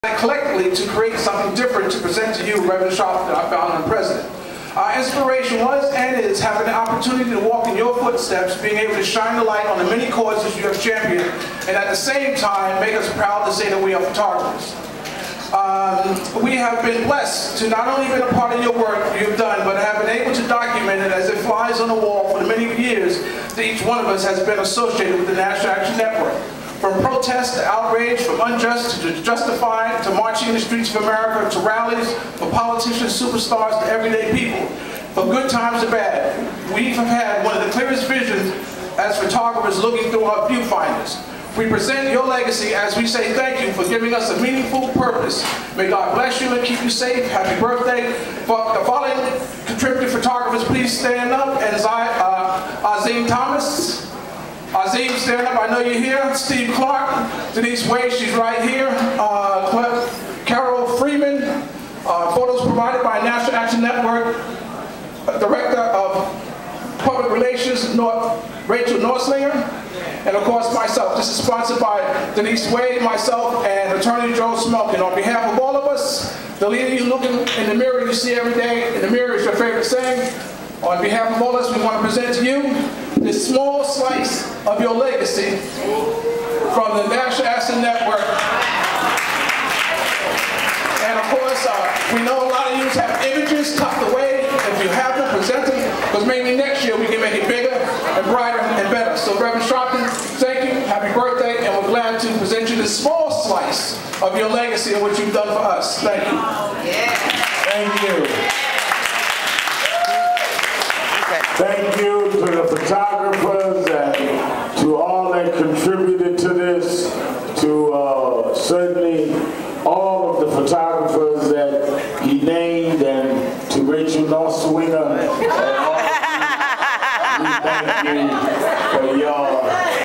Rev Al Sharpton's Collage Mural Birthday Gift on SGIBmagOnTV/The GEE Spot
For his 56th birthday (October 2010), the photographers of the National Action Network have decided collectively to create something different to present to you Rev. Doc Al Sharpton, Founder and President, a collage mural a photographic journey of his work presented by Gerard. The following contributing photographers are: Azim Thomas, Steve Clark, Denise Wray, Carol Friedman, photos provided by NAN Public Relations Director Rachel Noerdlinger and Photos By Gerard. The leader you're looking for you see every day in the mirror" Rev. Doc. Al Sharpton.